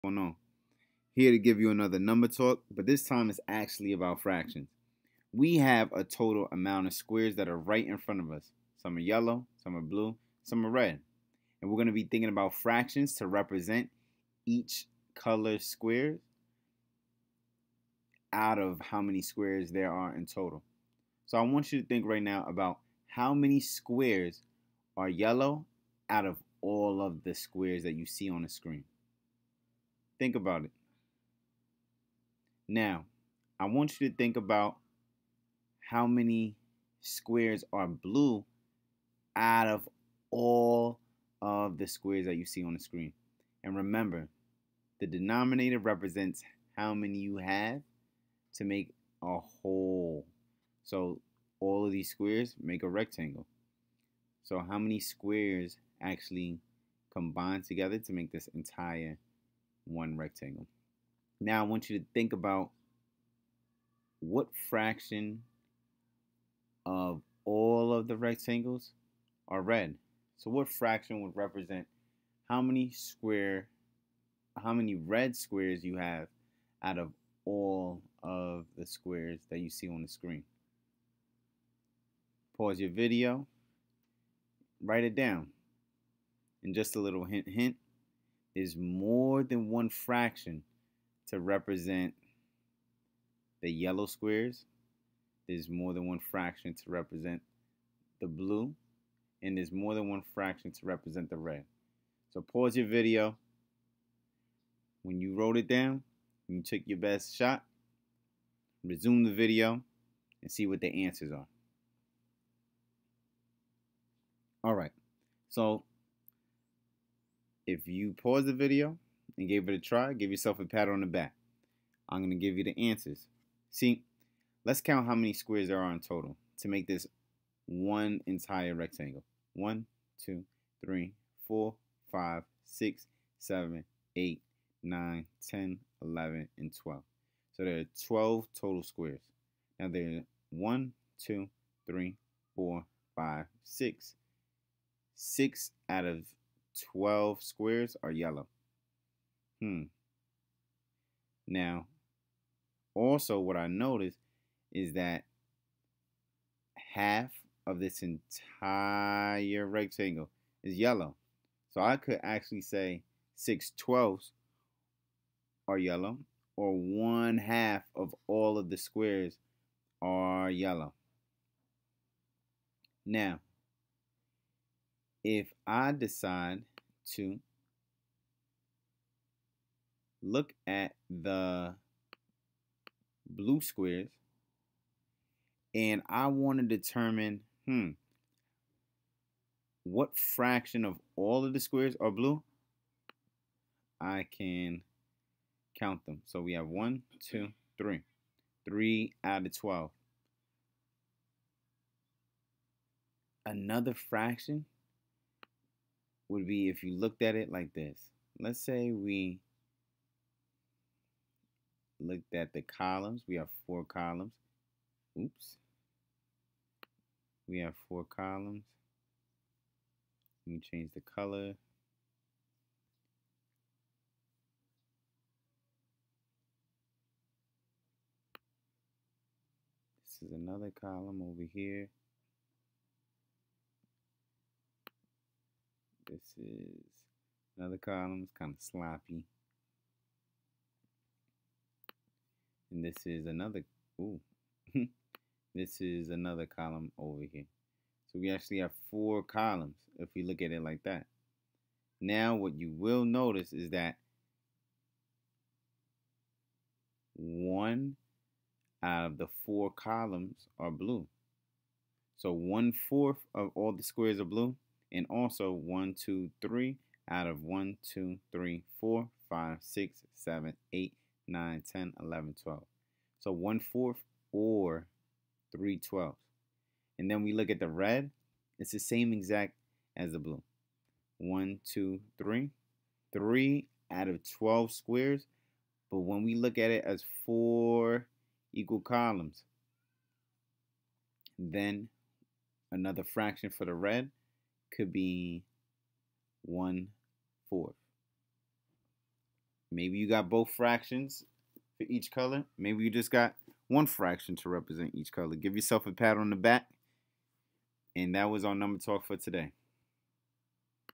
Fraction talk! Part 1
Can you identify a fraction to represent the amount of yellow squares out of all the squares? Red squares out of all the squares? Blue squares out of all the squares?

Challenge: Can you think of most then one fraction to represent the yellow, blue and red squares?